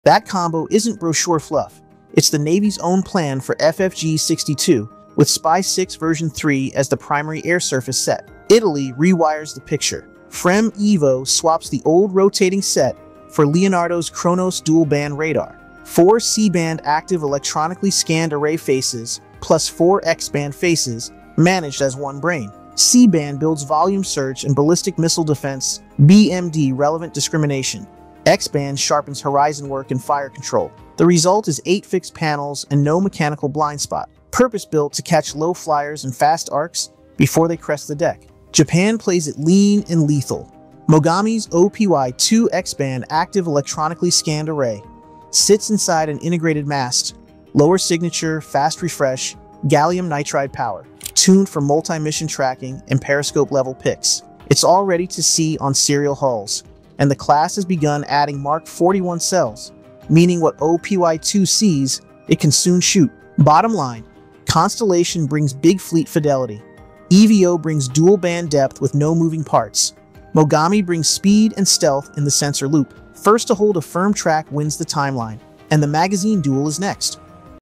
Naval Radar Showdown: SPY-6 vs Kronos vs OPY-2
Modern warships are becoming floating sensor networks—and radar is the game-changer. In this deep dive, we break down the next-gen arrays redefining sea control: the US Navy’s FFG-62 (Constellation-class) with SPY-6, Italy’s FREMM EVO with Leonardo’s Kronos family, and Japan’s Mogami-class featuring the OPY-2 X-band array. We’ll unpack how integrated masts, high-power AESA, and true multi-mission modes (air, surface, and ballistic-missile defense) are reshaping the fight.

What you’ll learn:
- How SPY-6 scales sensitivity and tracks more threats, farther
- Why FREMM EVO’s Kronos suite packs flexibility into an integrated mast
- Where Mogami’s OPY-2 X-band shines for sea-skimming and cluttered littorals
- Power, cooling, and software—the hidden enablers behind “smart” ships
- Counter-tactics and vulnerabilities: saturation raids, ECM, and decoys